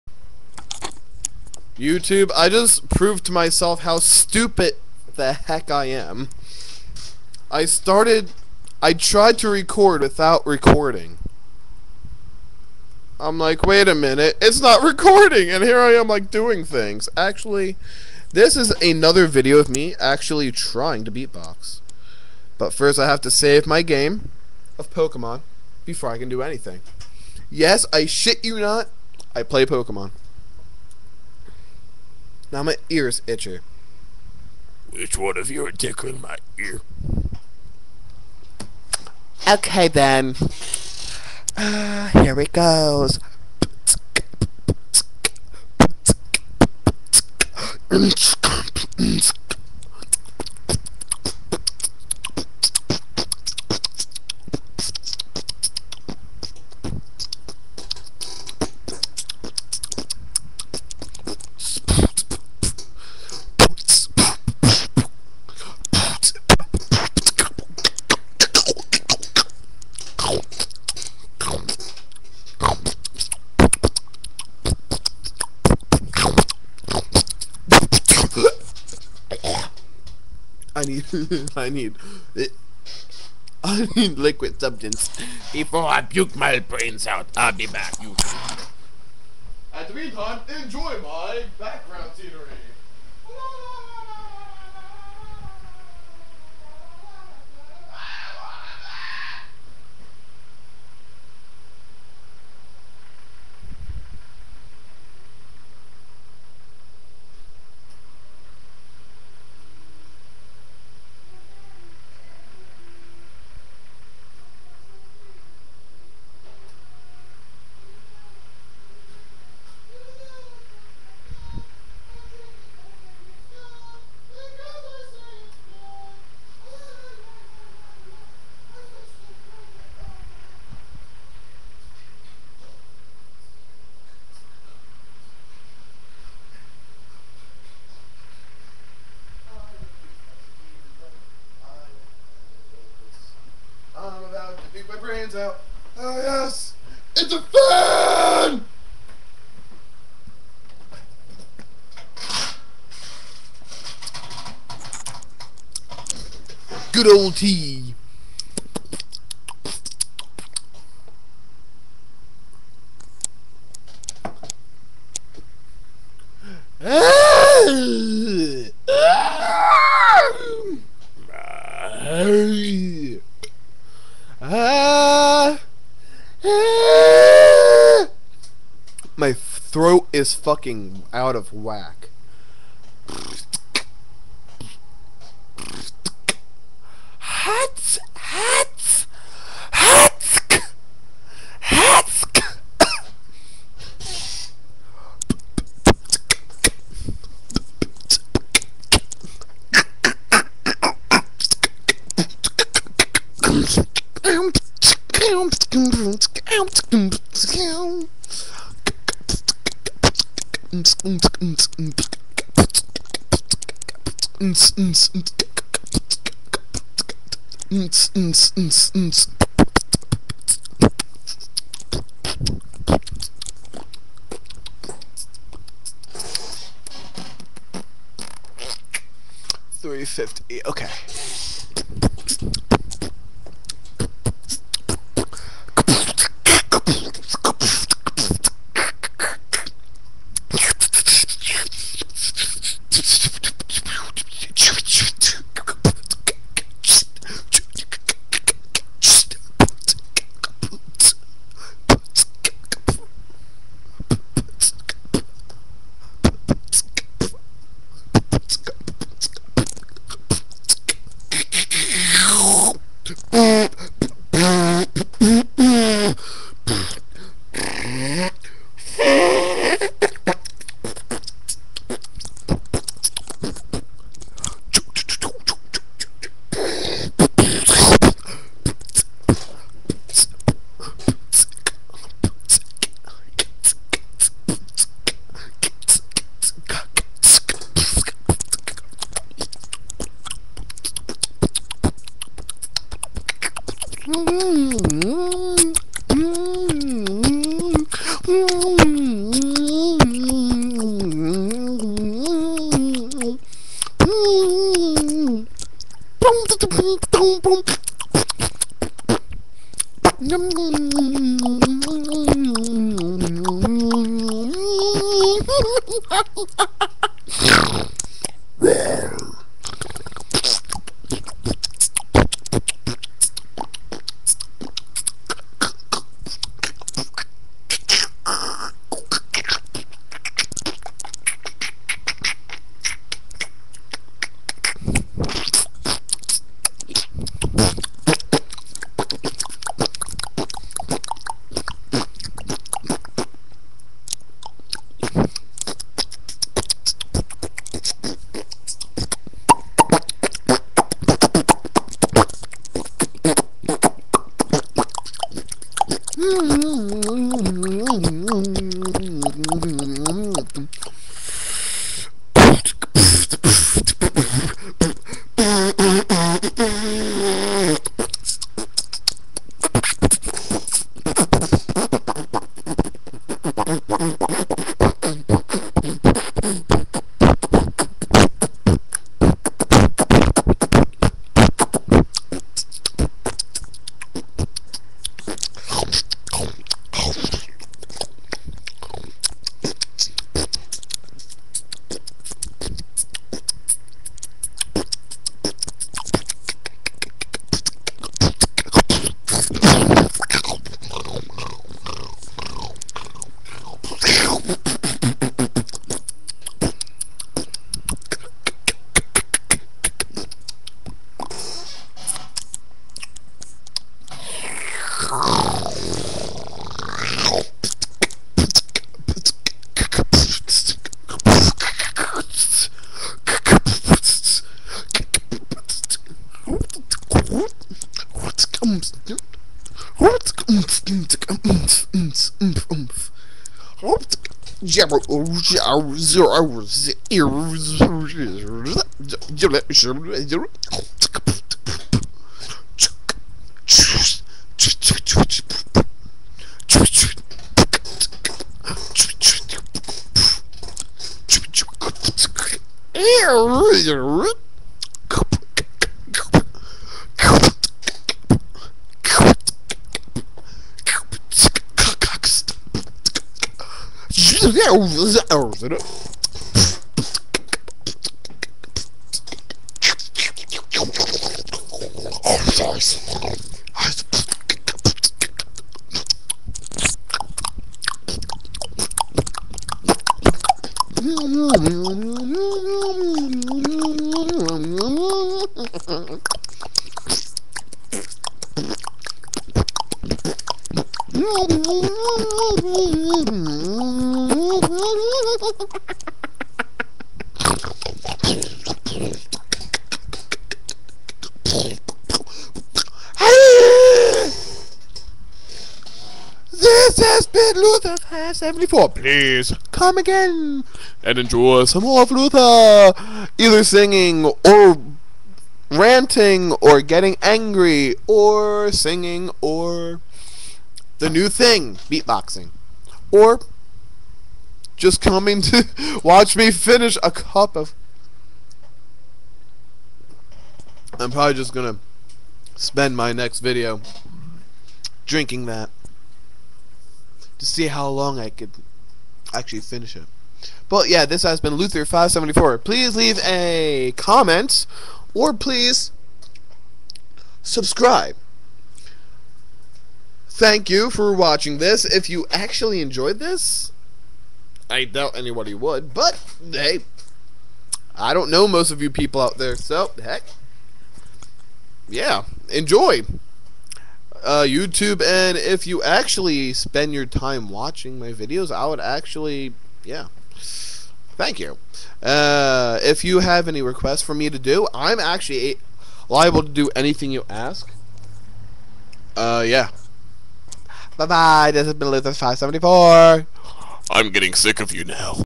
beatbox
Luther574's webcam video June 13, 2011 07:26 PM
I hape you enjoyed me sucking XD lol
at 1:28 you might wana skip I got up to get a drink but I was quite funny but  just easy to skip over to 2:26 ^_^ enjoy